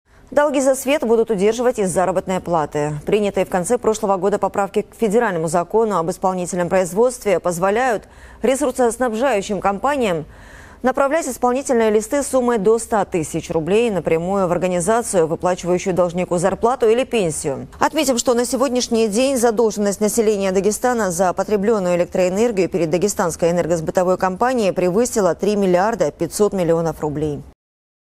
Долги будут удерживать из зарплаты!!!
Принятые в конце прошлого года поправки к Федеральному закону «Об исполнительном производстве» позволяют ресурсоснабжающим компаниям направлять исполнительные листы суммой до 100 тысяч рублей напрямую в организацию, выплачивающую должнику зарплату или пенсию.

Отметим, что на сегодняшний день задолженность населения Дагестана за потребленную электроэнергию перед «Дагестанской энергосбытовой компанией» превысила 3 млрд 500 миллионов рублей